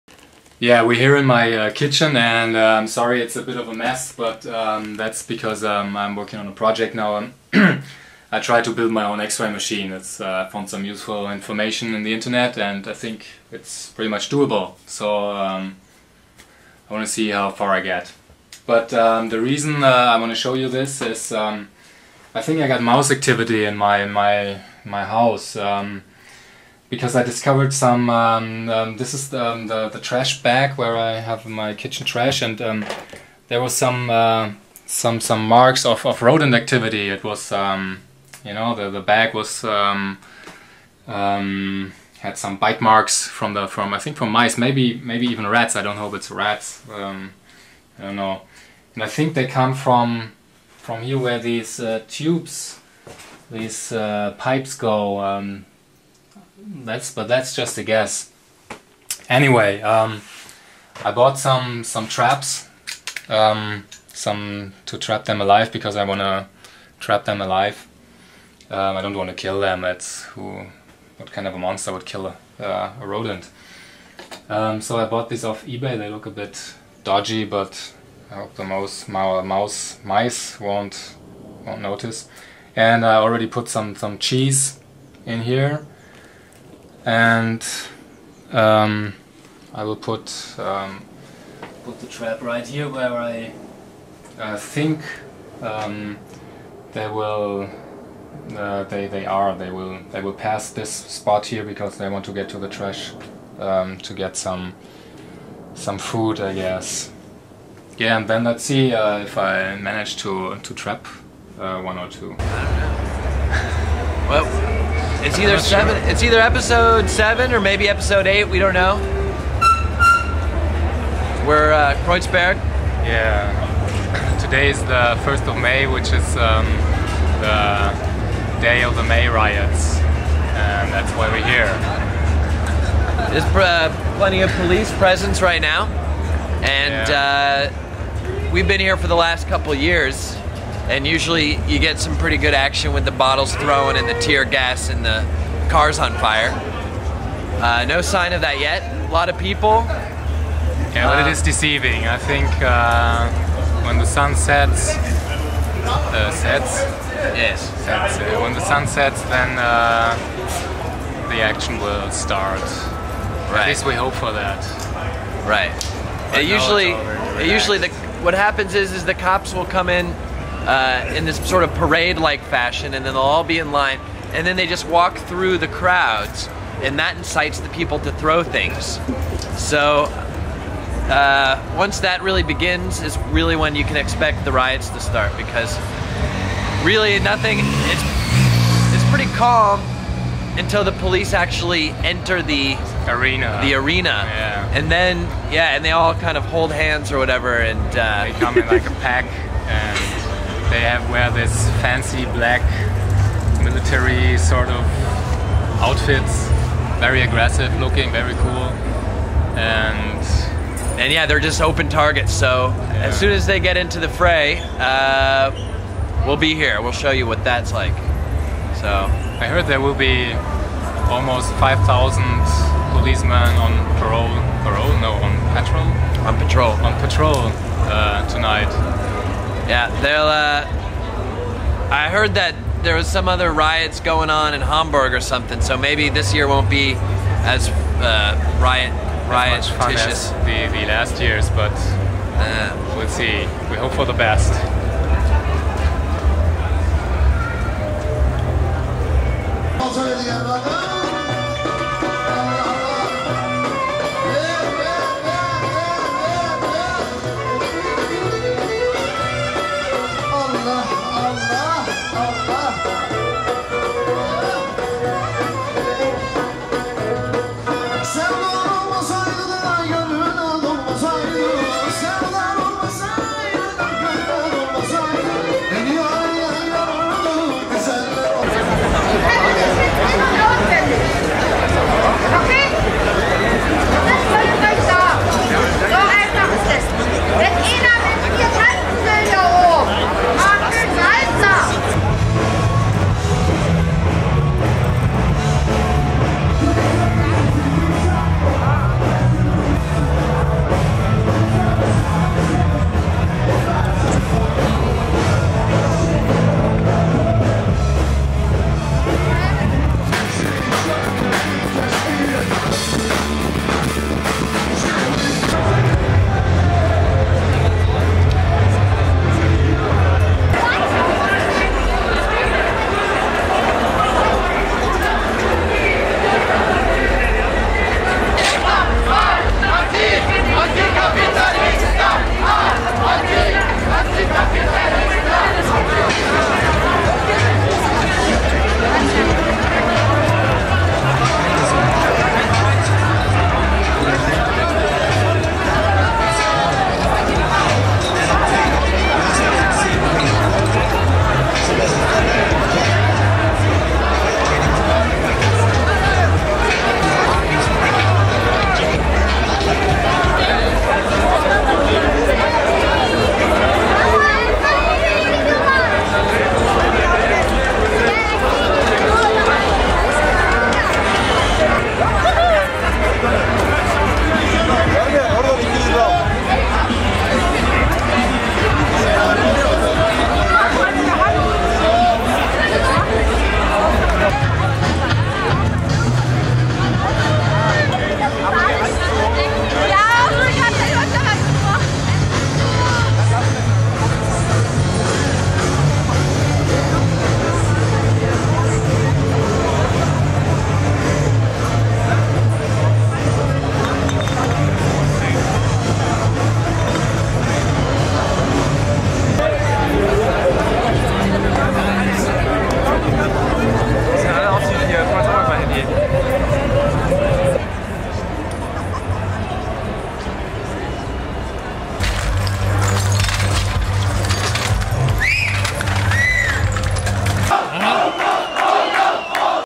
First We Take Berlin - Ep. 8.1 - May Riots in Kreuzberg 1.1
Thorsten has a problem with rodents at his home. After he tells us all about it we go to the traditional may riots that go on in Kreuzberg every first of may since more than 20 years now. We start at the MyFest where lots of bands play and then go right into the action when the riots finally begin. Poor Katie has not so much fun as we have.